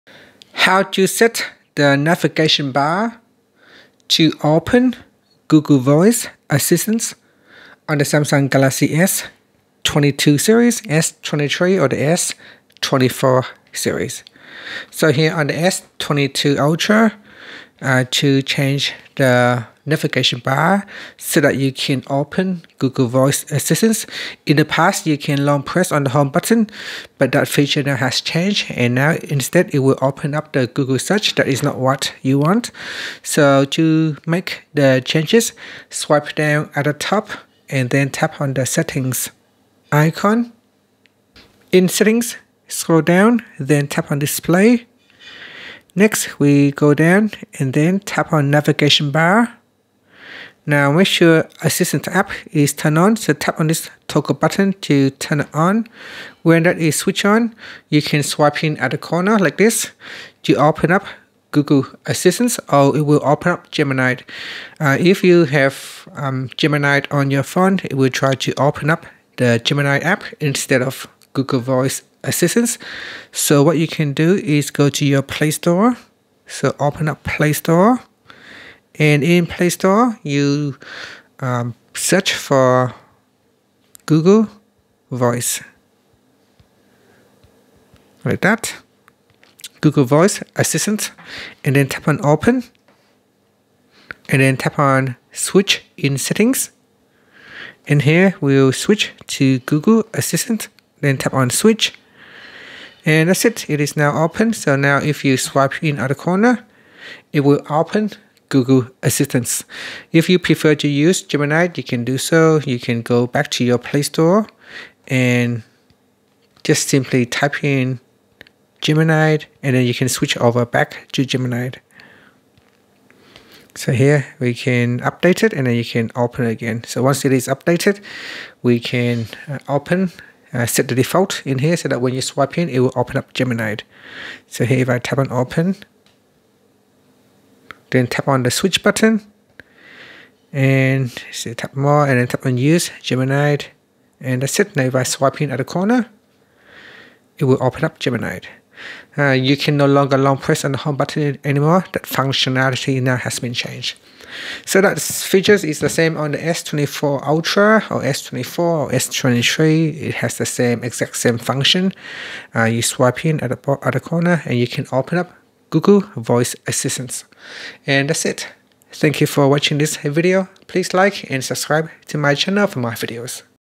Galaxy S24/S23/S22: How to Open Google Voice Assistant In Navigation Bar (New 2024 Update)
In this video, I will show you how to Open Google Voice Assistant In Navigation Bar on the Galaxy S24/S24+/S24 Ultra with this new Android update 2024. This tutorial also works with Galaxy S23, and S22.